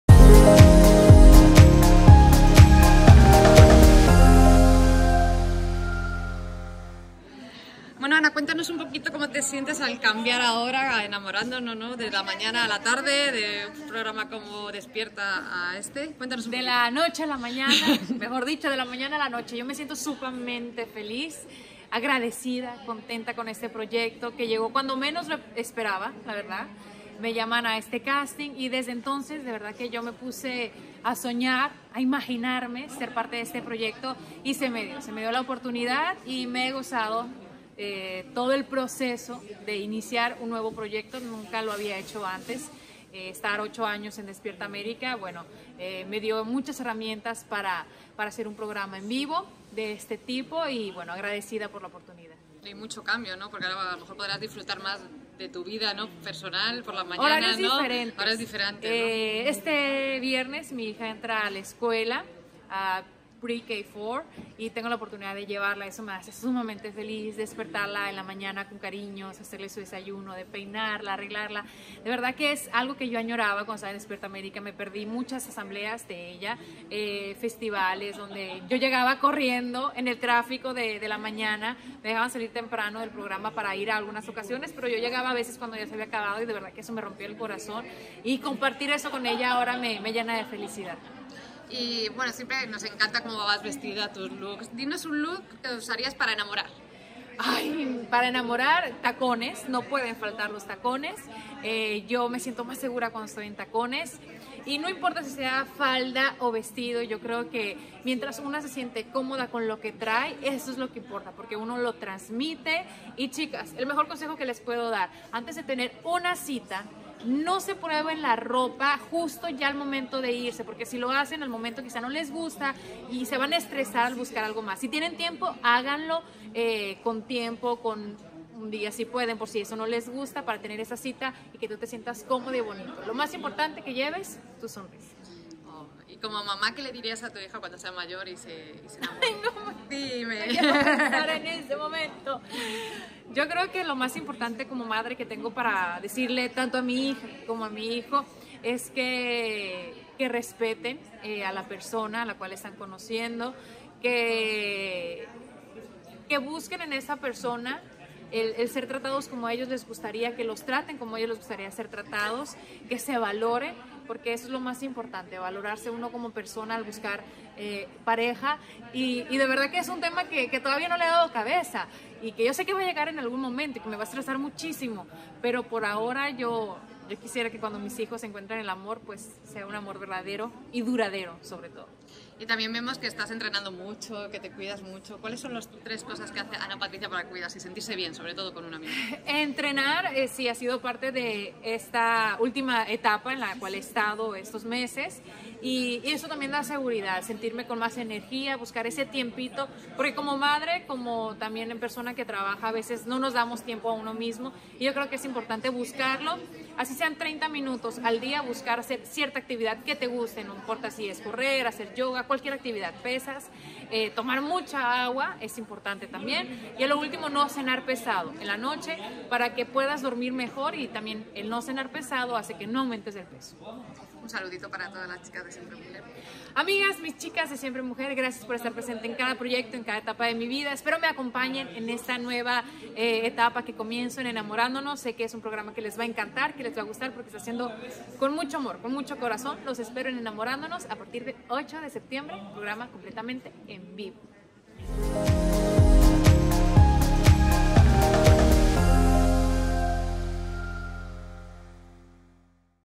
Ana Patricia habla de su salida de “Despierta América” y su nueva etapa en “Enamorándonos"
Ana Patricia Gámez volvió a Univisión, después de su salida de “Despierta América”, como conductora junto al chileno Rafael Araneda del nuevo programa del amor de Unimás, “Enamorándonos".